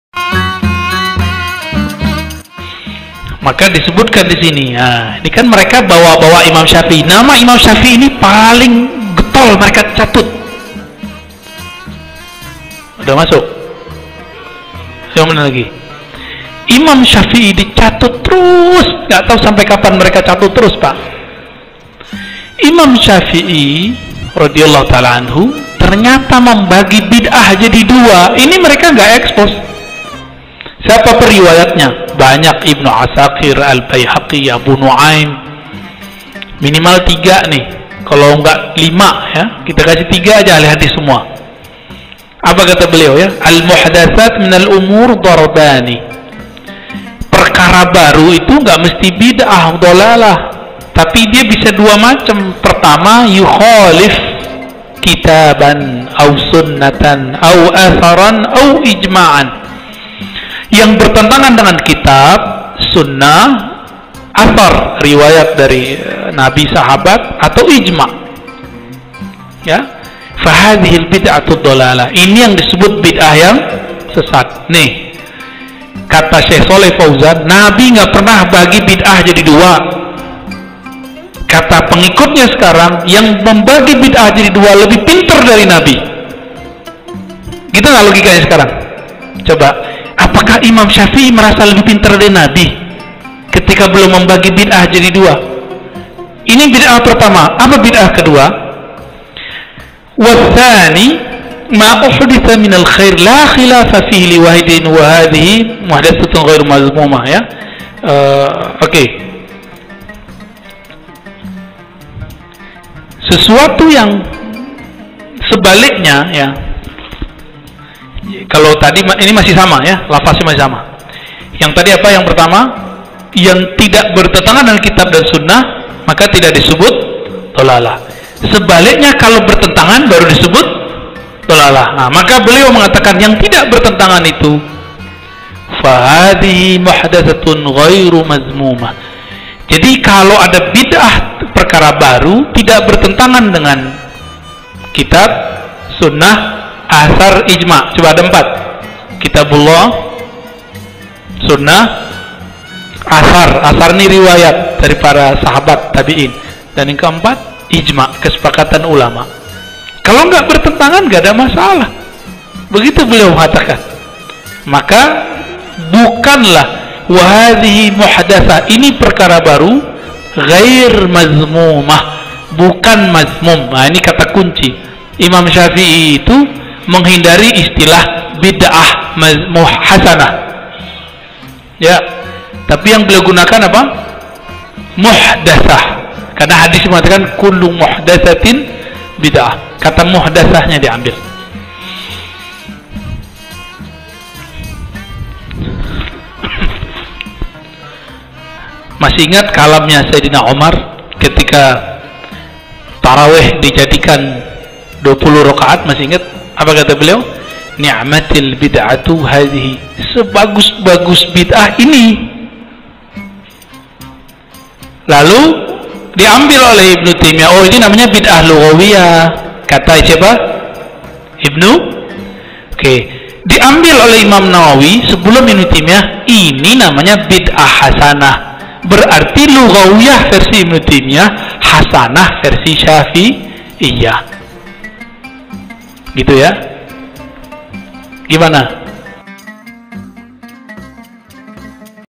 PEMBAGIAN BID'AH MENURUT IMAM SYAFII
PEMBAGIAN BID'AH MENURUT IMAM SYAFII ‎@JALAN TERBUKA

Imam Syafi'i berkata bidah itu terbagi dua. Bid'ah mahmudah (terpuji) dan bid'ah Madzmumah (tercela). Jika sesuai dengan sunnah, maka bid'ah mahmudah, namun jika bertentangan dengan sunnah, maka itu itu bid'ah madzmummah.